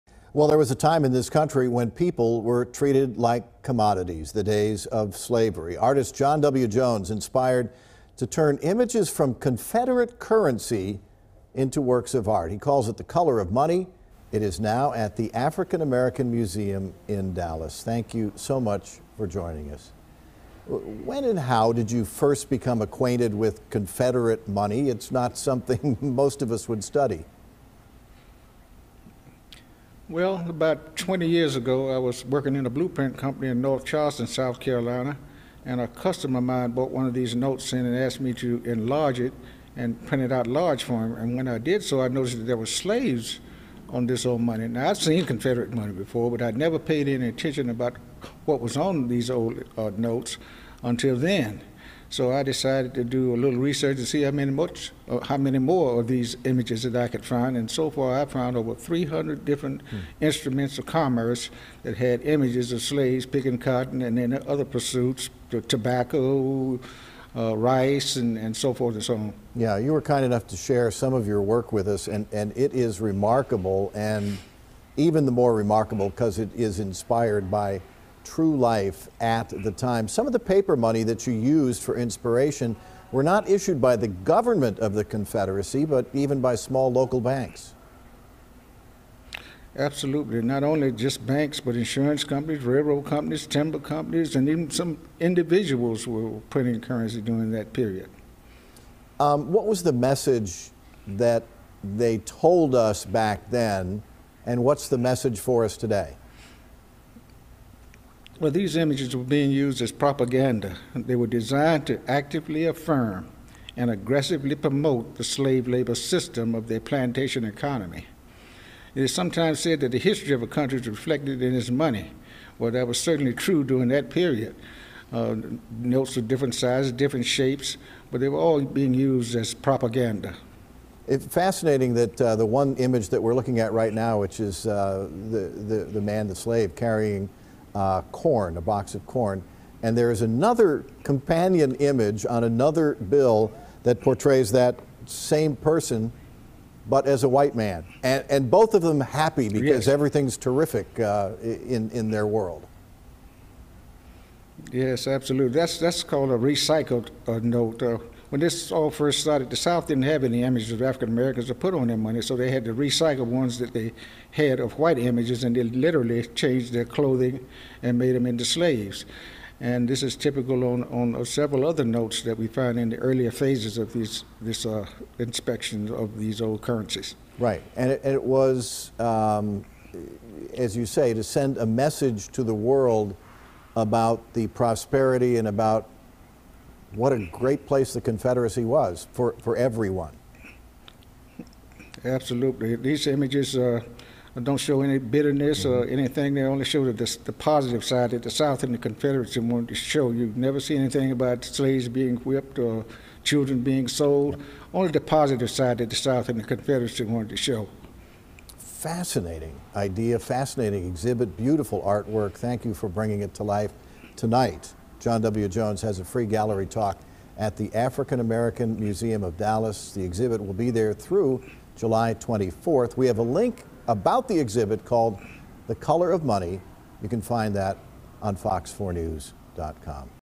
Artist turns Confederate money into works of art
There was a time in this country when people were treated like commodities -- the days of slavery. Artist John W. Jones was inspired to turn images from the Confederate currency into art pieces that are now on display at the African American Museum in Dallas.